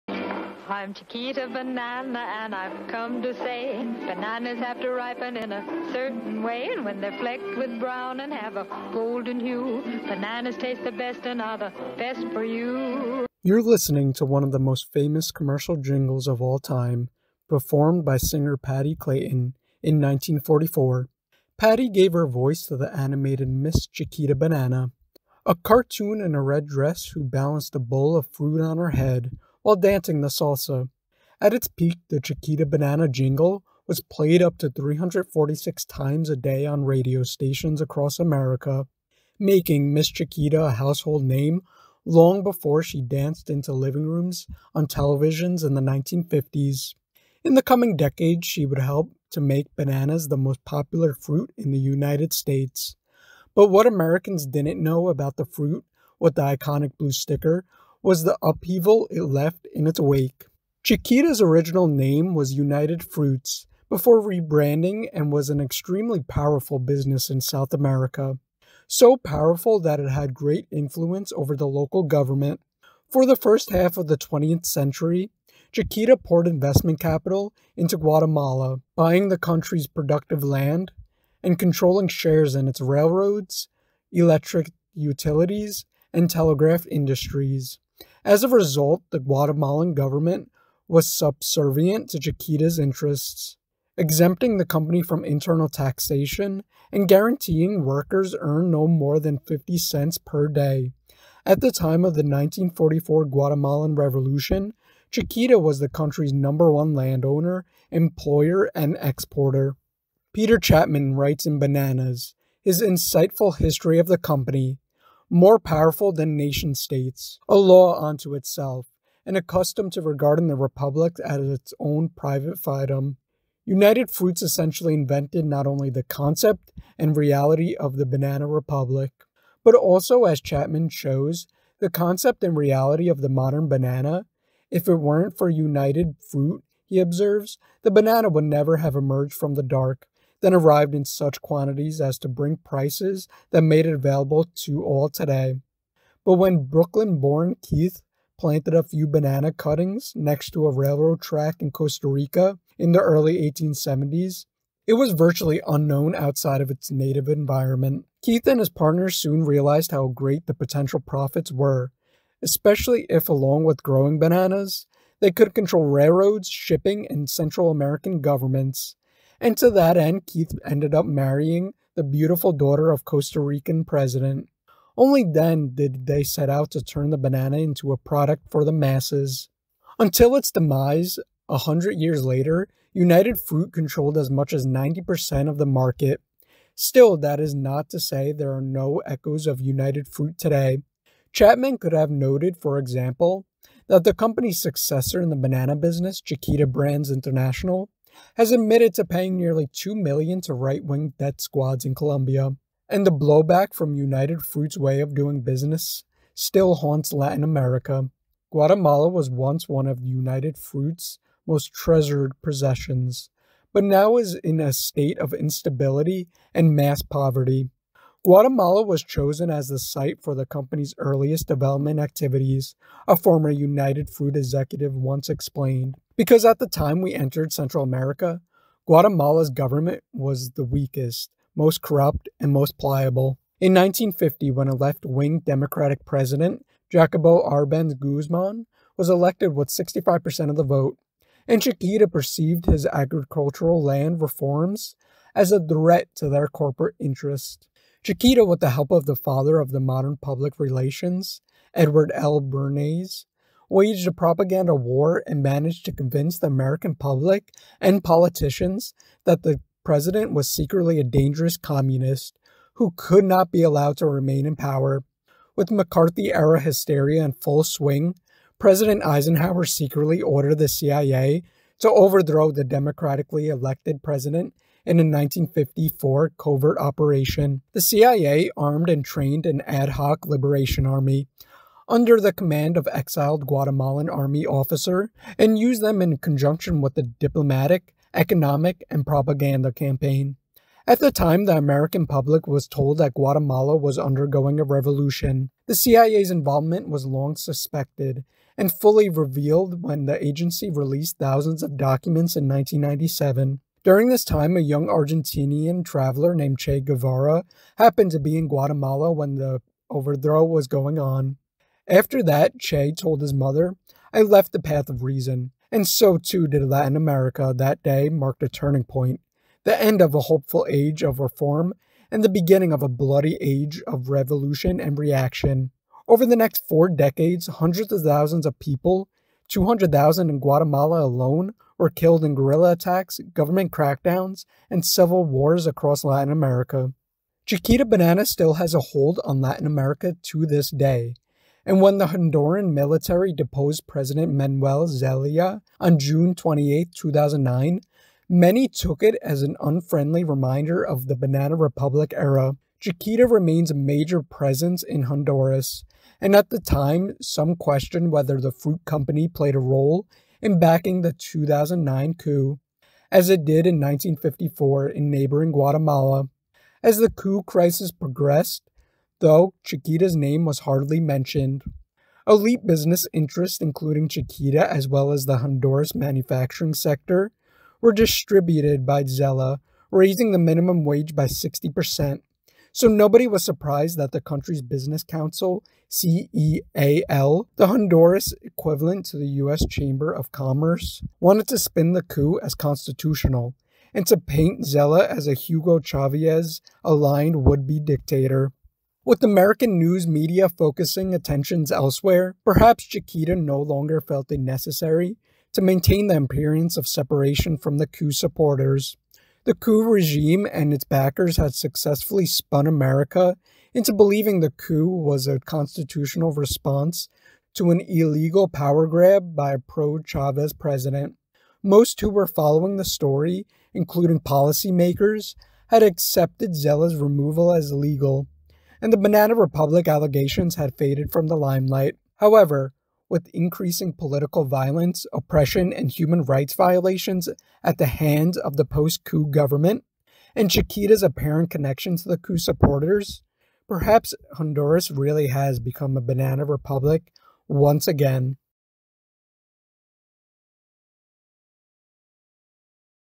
The Dark History of Chiquita Banana
Chiquita’s original name was United Fruits before rebranding and was an extremely powerful business in South America; so powerful that it had great influence over the local government. For the first half of the 20th century, Chiquita poured investment capital into Guatemala, buying the country's productive land and controlling shares in its railroad, electric utility, and telegraph industries; as a result, the Guatemalan government was subservient to Chiquita's interests, exempting the company from internal taxation and guaranteeing workers earned no more than fifty cents per day. At the time of the 1944 Guatemalan revolution, Chiquita was the country's number one landowner, employer, and exporter.